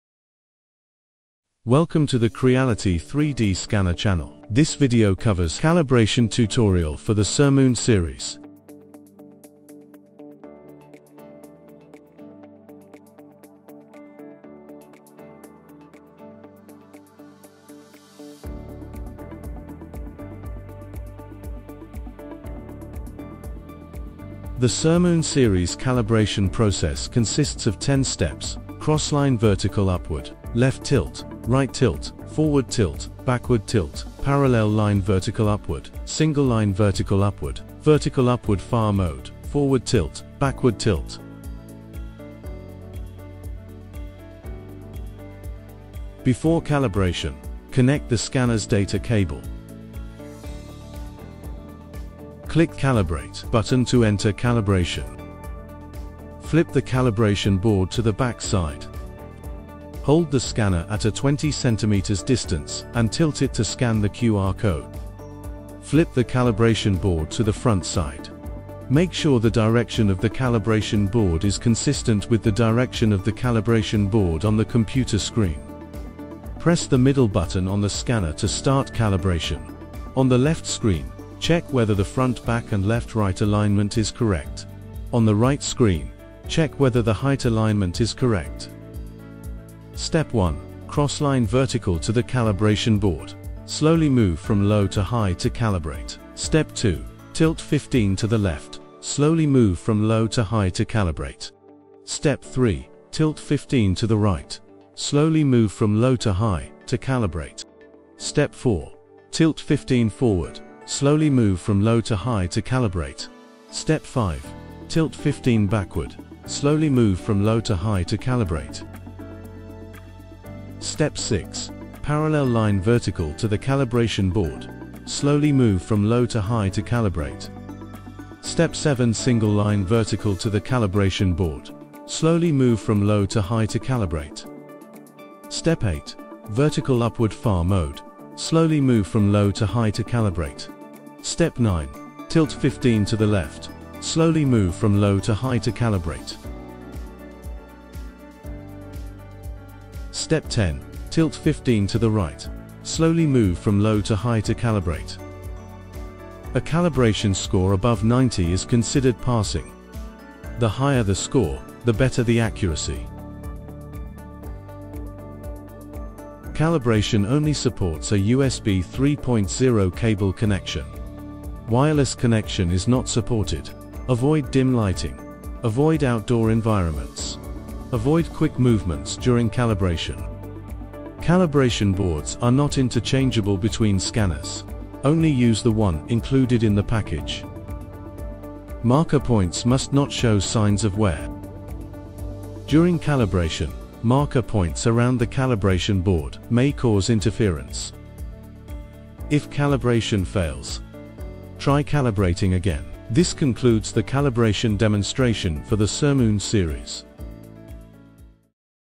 Creality Sermoon Series 3D Scanners Calibration Tutorial
This video walks you through the calibration process for the Creality Sermoon series 3D scanners.

You’ll learn:
* When and why calibration is required
* Step-by-step instructions for accurate calibration
* Key tips and precautions to maintain top-level scanning precision